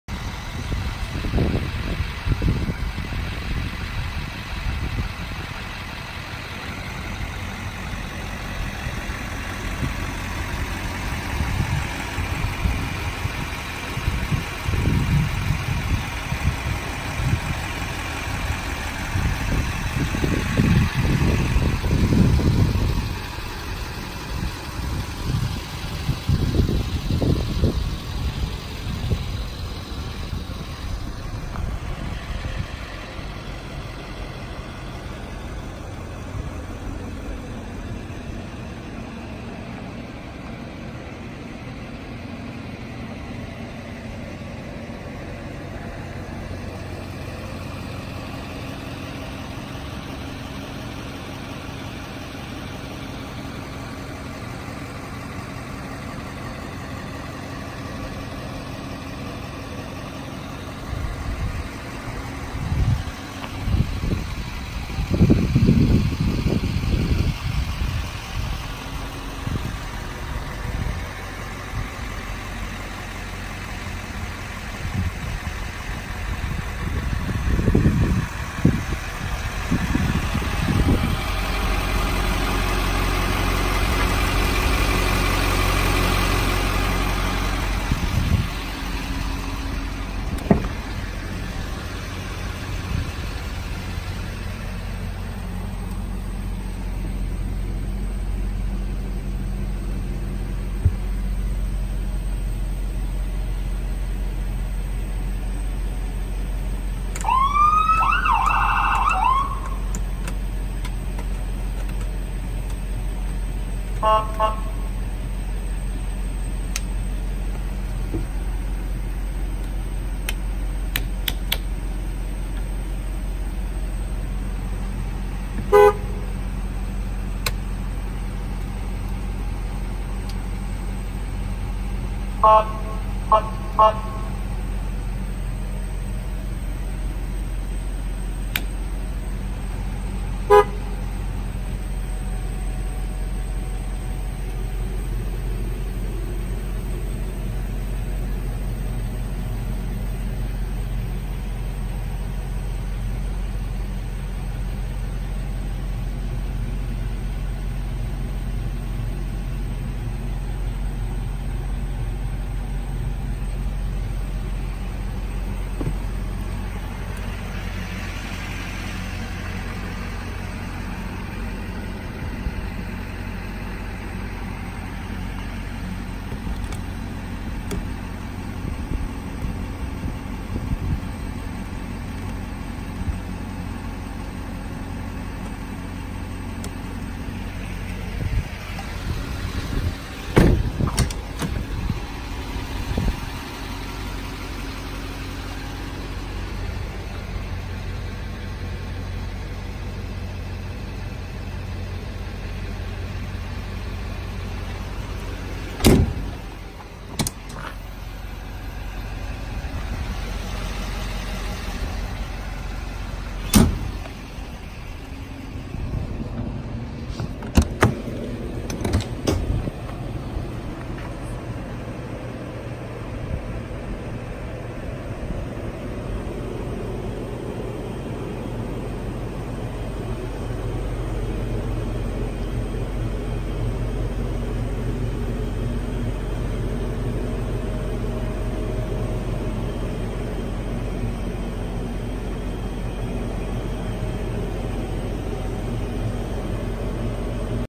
AEV Ambulance for sale by Pilip Customs
2012 Chevrolet G4500 AEV Type 3 Ambulance

For sale by Pilip Customs
We buy and sell used ambulances.

LED emergency light package.  Fully loaded ambulance prep package.